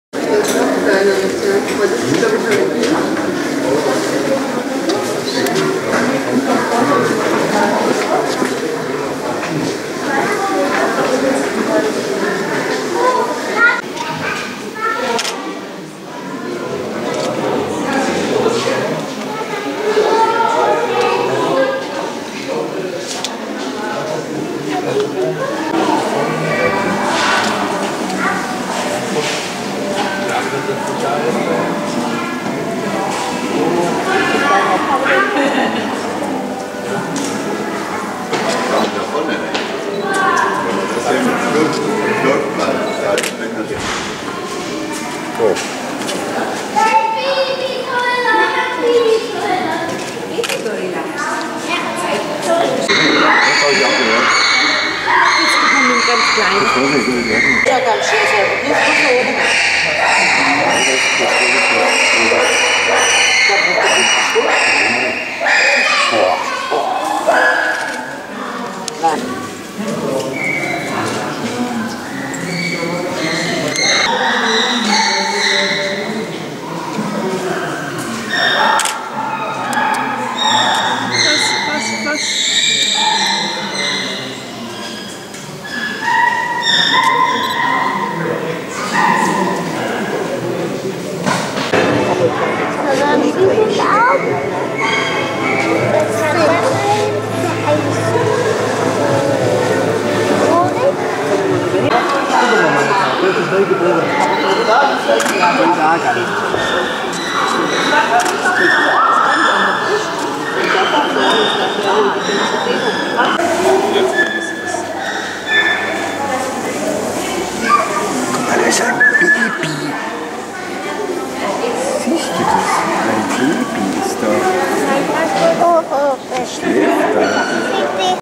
Zoo Stuttgart - Wilhelma: Gorillababy Masani (von Mutasi) - 3 Tage alt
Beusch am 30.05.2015
Masani wurde am 27.05.15 geboren